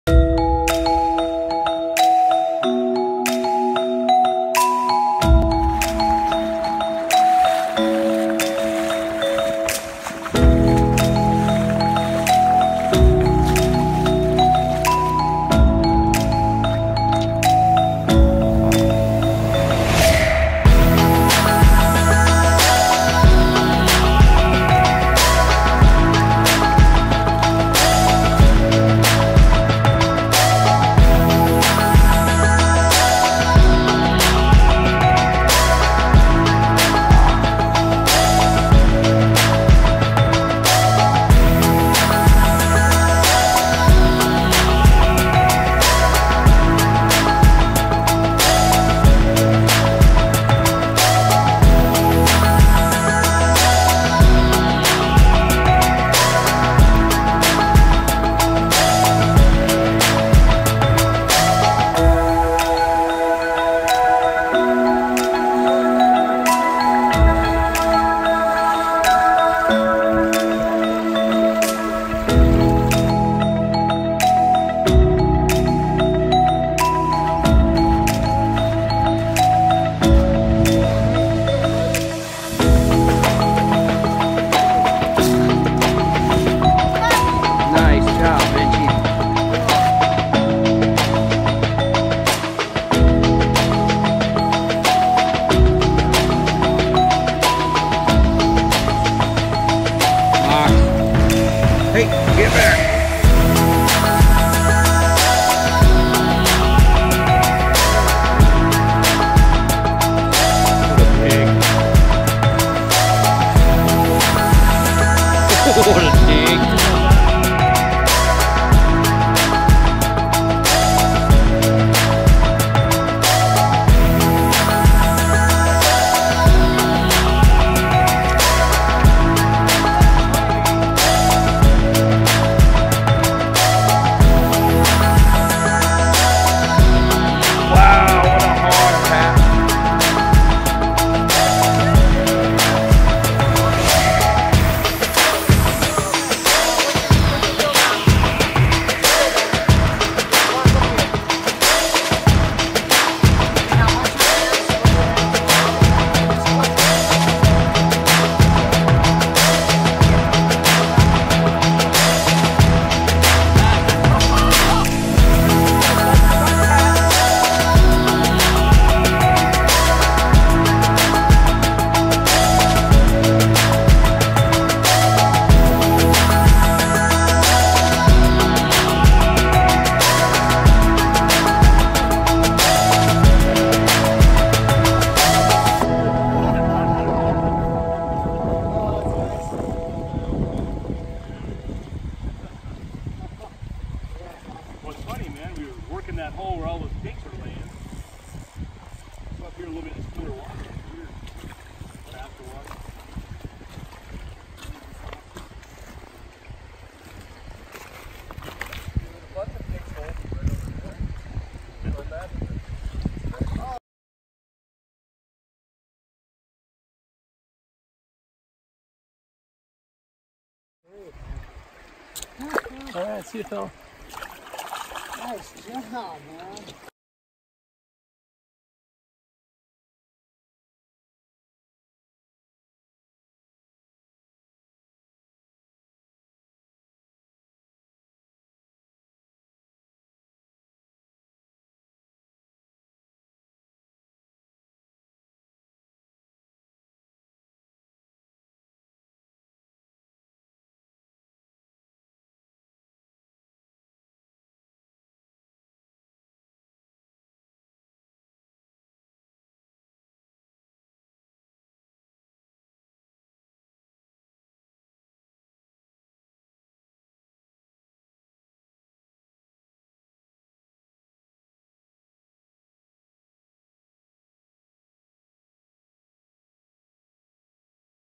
Epic fishing for Alaska’s native Rainbow trout
Fly fishing and rafting in Alaska!!  Join Ak Wild River Adventures as we recap the beautiful native rainbows we got to meet the summer of 2019.